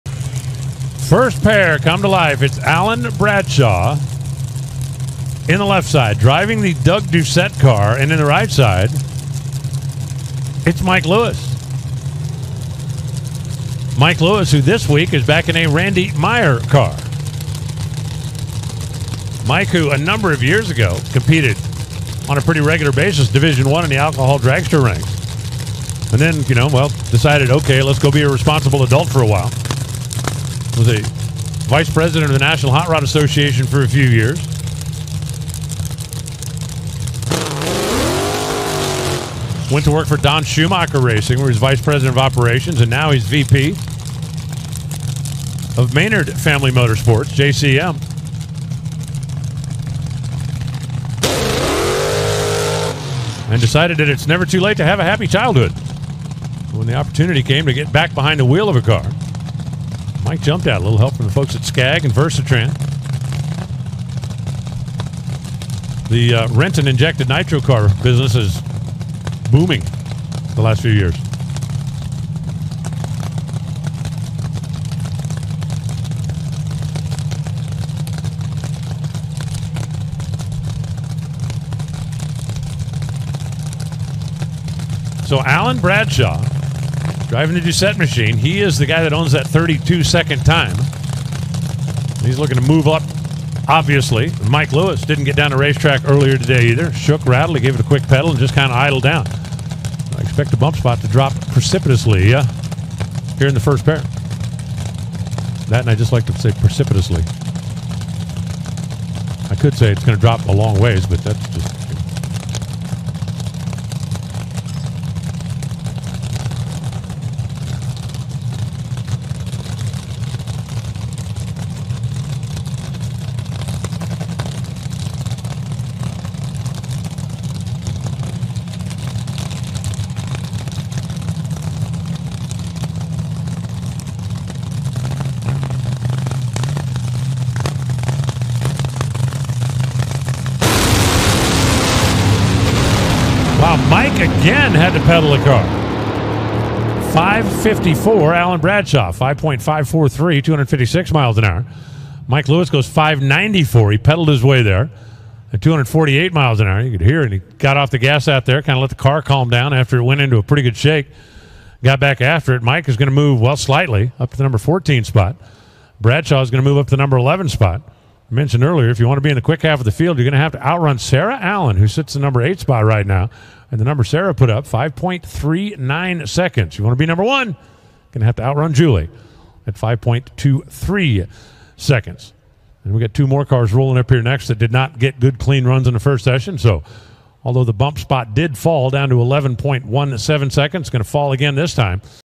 Alan Bradshaw 5 543 256 26, Mike Lewis 5 949 248 93, Top Alcohol Dragster, Qualifying Rnd 2, ZMAX Dr
Alan Bradshaw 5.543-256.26, Mike Lewis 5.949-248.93, Top Alcohol Dragster, Qualifying Rnd 2, ZMAX Dragway, 15th annual betway Carolina Nationals Concord NC. Sep 22, 2023